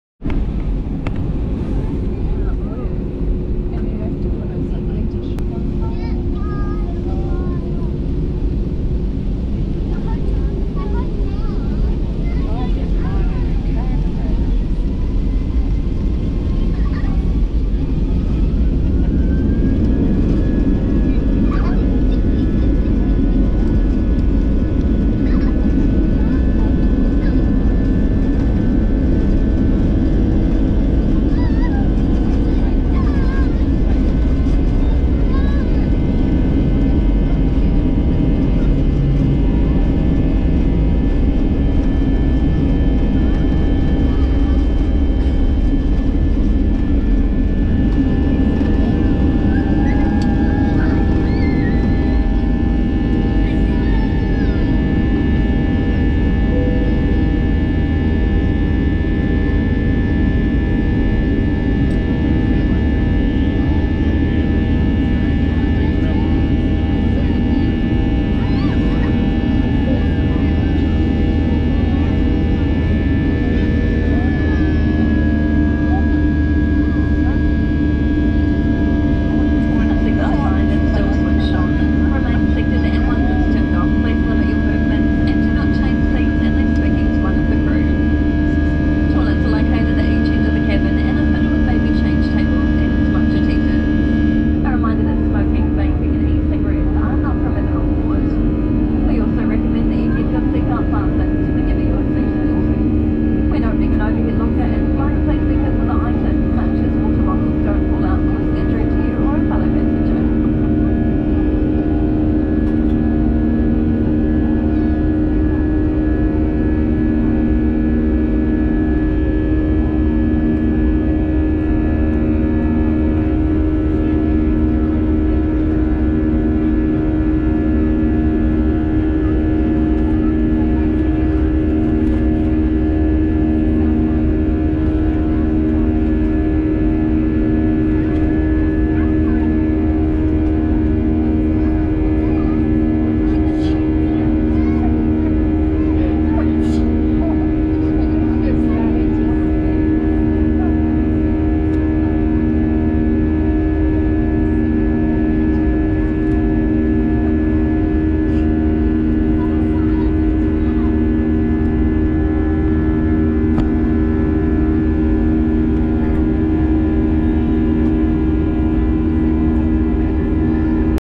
Auckland Airport Takeoff on a Clear Day | Jetstar A320-200 | Flight to Christchurch
Join us for an exhilarating takeoff from Auckland Airport on a clear day as we embark on a journey to Christchurch aboard a Jetstar A320-200. Experience the thrill of acceleration and the stunning aerial views of New Zealand's breathtaking landscapes. Sit back, relax, and enjoy this captivating flight experience!